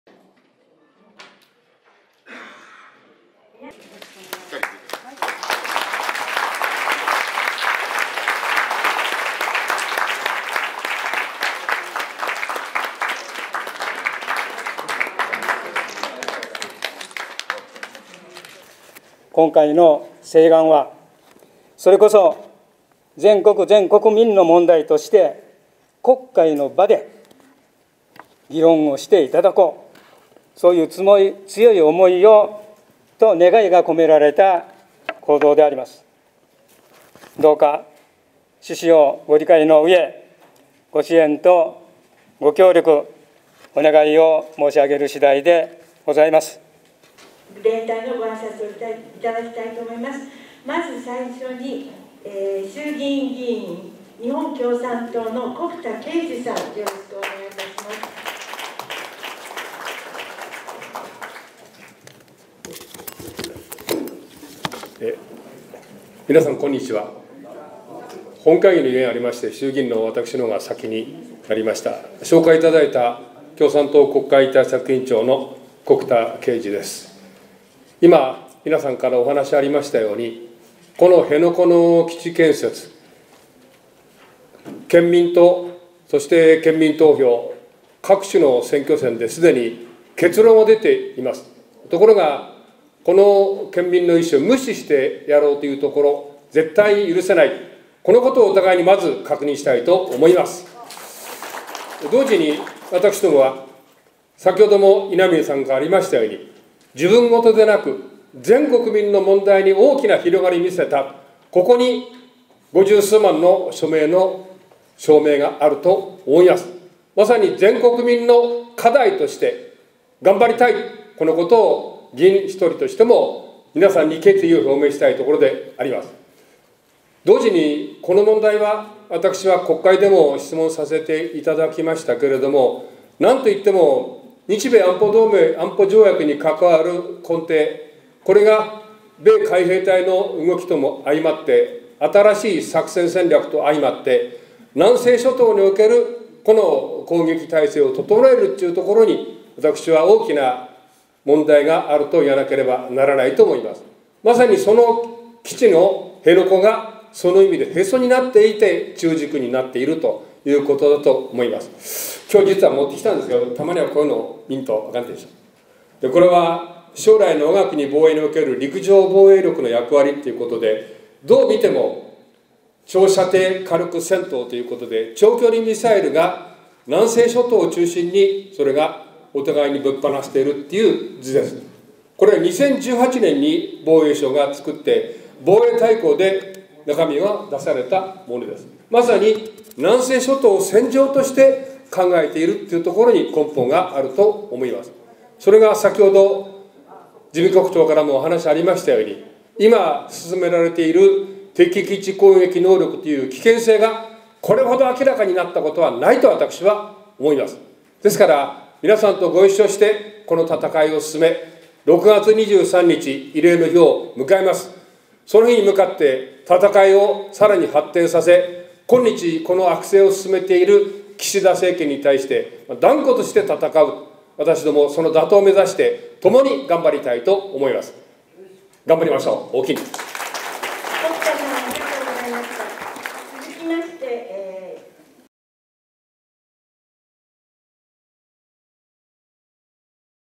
559,224筆 辺野古基地建設断念求める署名提出 2023.6.13
穀田恵二 国対委員長・衆院議員あいさつ

辺野古新基地建設断念を求める請願署名提出集会

主催/オール沖縄会議・国会請願署名実行委員会